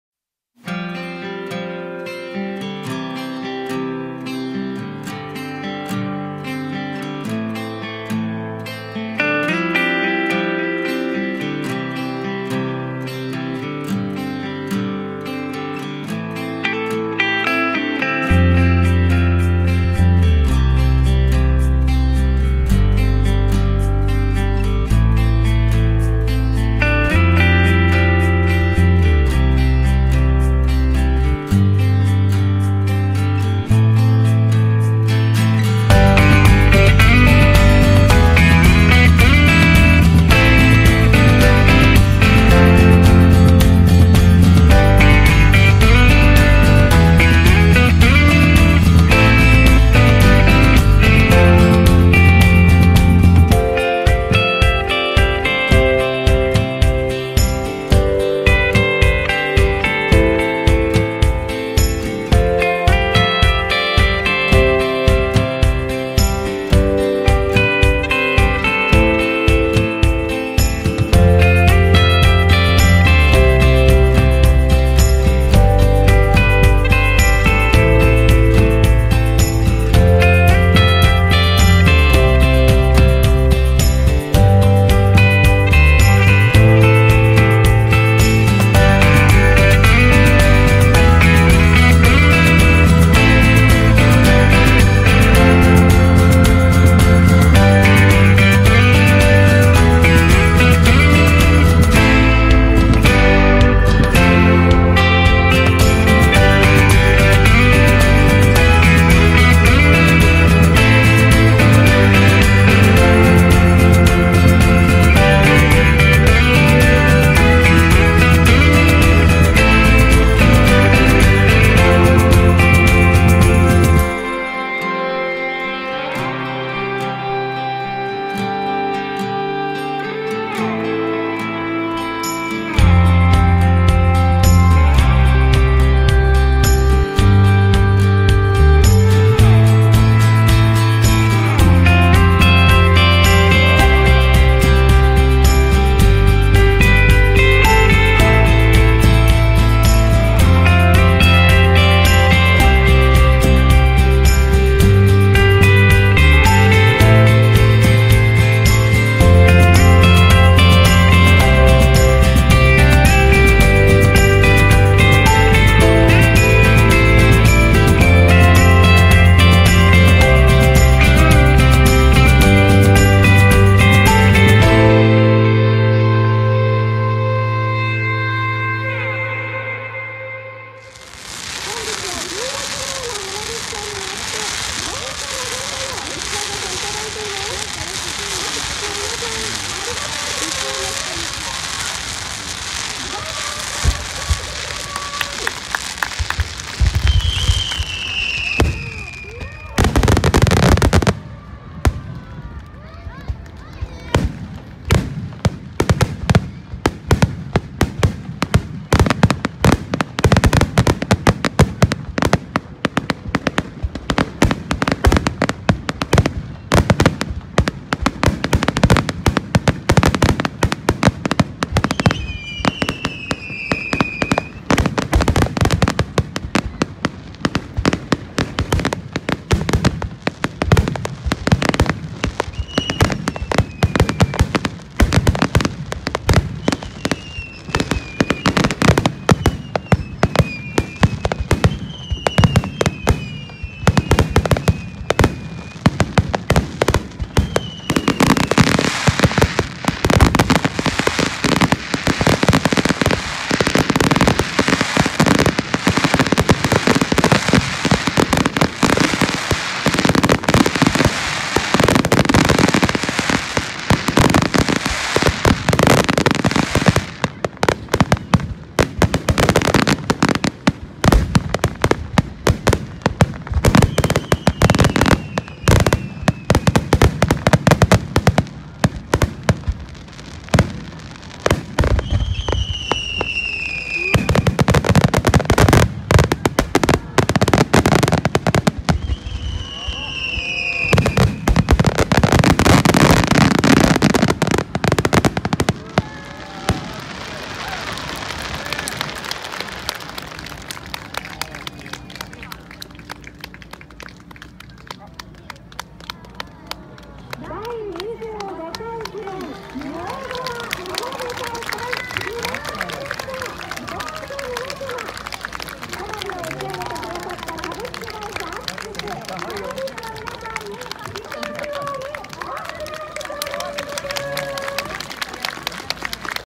第25回根尾川花火大会GoPro HERO5sessionでタイムラプス
2018年8月11日第25回根尾川花火大会に行ってきました。
GoproHero５Sessionでタイムラプス動画で撮ってみました。